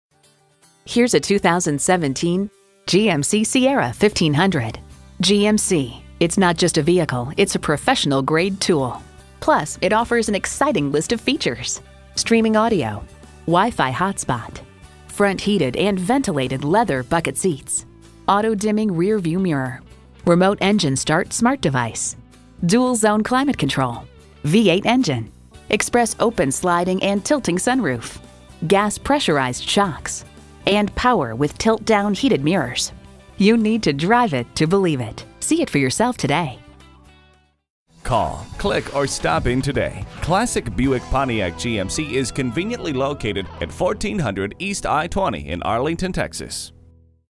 Used 2017 GMC Sierra 1500 Arlington TX Fort Worth, TX
Year: 2017
Make: GMC
Model: Sierra 1500
Trim: SLT
Engine: 5.3l 8 Cylinder Fuel Injected
Transmission: 2WD Automatic
Color: White
Interior: Jet Black
Mileage: 16829
Stock #: HG330453
VIN: 3GTP1NEC4HG330453
Summit White 2017 GMC Sierra 1500 SLT RWD 6-Speed Automatic Electronic with Overdrive V8 6-Speed Automatic Electronic with Overdrive, jet black Leather, ABS brakes, Alloy wheels, Chrome Door Handles, Compass, Dual-Zone Automatic Climate Control, Electronic Stability Control, Emergency communication system, Front dual zone A/C, Heated door mirrors, Heated Driver & Front Passenger Seats, Heated front seats, Illuminated entry, Low tire pressure warning, Power Adjustable Pedals, Power Folding & Adjustable Heated Outside Mirrors, Rear Vision Camera, Remote Keyless Entry, Remote keyless entry, Remote Vehicle Starter System, Single Slot CD/MP3 Player, Traction control, Trailering Equipment.Odometer is 10065 miles below market average!Awards: * 2017 KBB.com Brand Image AwardsReviews: * Strong combination of fuel economy and power; comfortable, quiet cabin; compliant ride quality makes it suitable for daily use; many available configurations and trims Source: Edmunds

Address:
1400 E-I20 HIghway